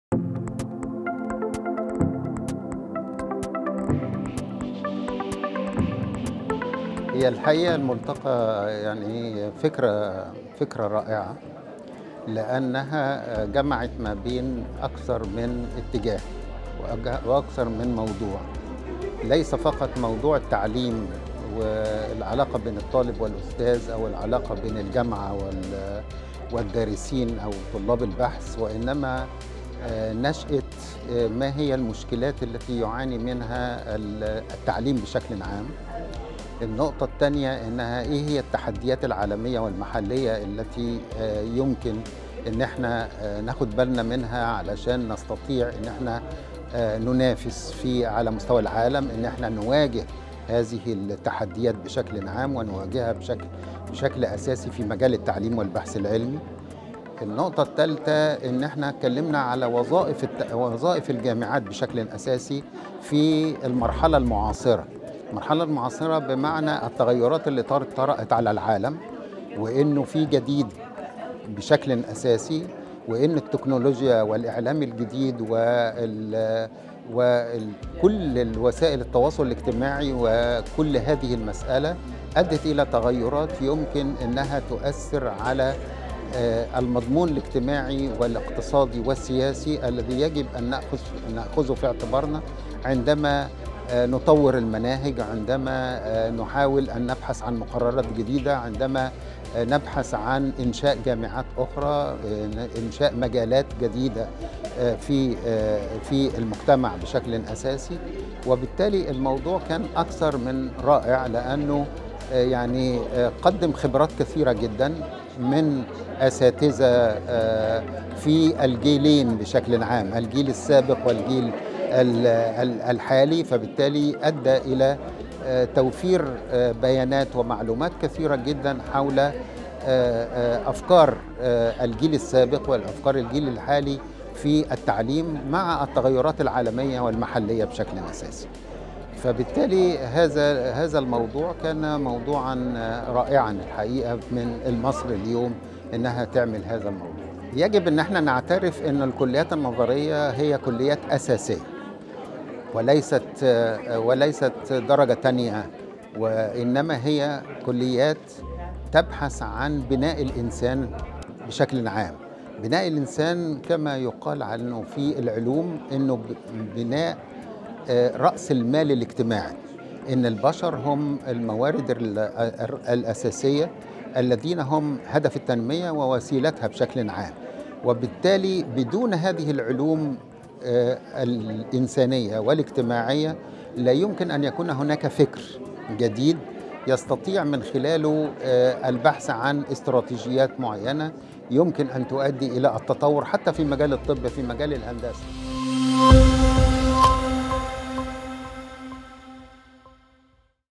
د.أحمد حجازي في ملتقى المصري اليوم: يجب على الجامعات استيعاب التطور في العالم من تغيير أدوات ومناهج
انطلقت فعاليات الملتقى_النوعي_الأول_للمصري_اليوم لبحث مستقبل_التعليم في مصر وفرص الوظائف في المستقبل، وأعلن الدكتور خالد عبد الغفار خلال جلسات الملتقى الذي شهد حضور عدد من الخبراء ومشاركة وزير التعليم العالي، وجود 12 جامعة أهلية جديدة، تدخل الخدمة أكتوبر المقبل بجانب 5 جامعات في العاصمة الإدارية الجديدة، وكشف الملتقى عن اختفاء 85 مليون وظيفة عام 2025، وظهور وظائف جديدة بناء المهارات، وأن مصر بها مهارات شابة يمكن تصديرها للخارج.

فيما ناقشت الجلسة الثانية والتي كانت بعنوان "جامعاتنا وجاهزيتها للعام الجديد ومحددات المنافسة الداخلية والخارجية فى التعليم العالى"، فيما كانت الجلسة الأخيرة تحت عنوان "عصف ذهني حول أسس النهوض بالجامعات لتواكب العصر وتوجيهات للطلبة الجدد تعينهم على حسن ودقة الاختيار".

وفي ختام الملتقى الذي أداره الأستاذ عبداللطيف المناوي العضو المنتدب ورئيس تحرير المصري اليوم كرمت مؤسسة المصري اليوم أوائل الثانوية العامة في مصر، بمشاركة عدد كبير من الخبراء ومؤسسات المجتمع المدني.